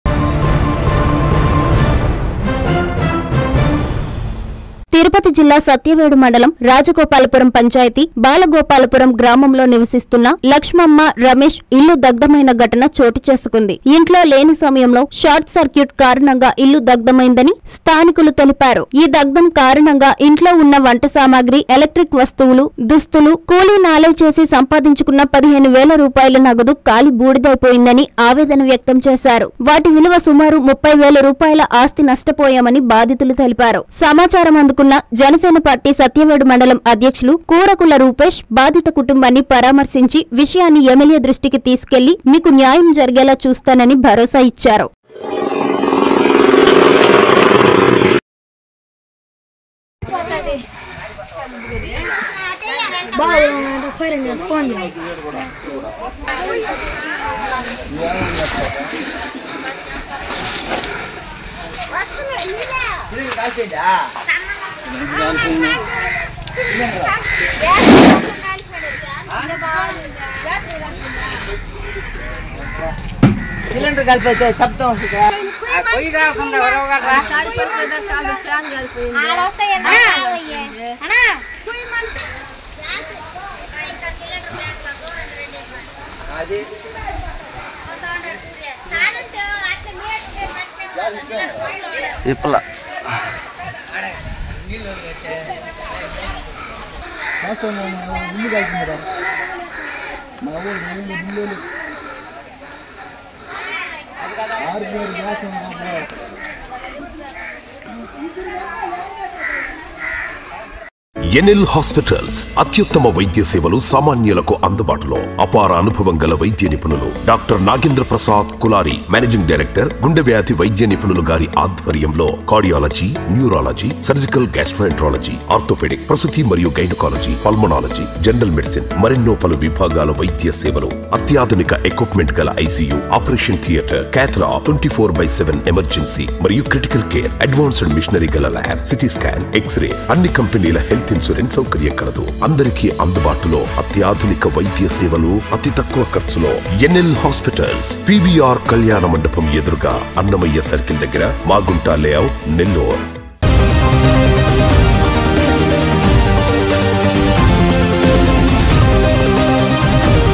త‌గ‌ల‌బెట్టారా...? త‌గ‌ల‌బ‌డిందా?
- ఇల్లు ద‌గ్ధం - రూ. 30వేలు ఆస్తి న‌ష్టం
- న్యాయం జరిగేలా చూస్తాం - జనసేన పార్టీ కూరకుల రూపేష్ హామీ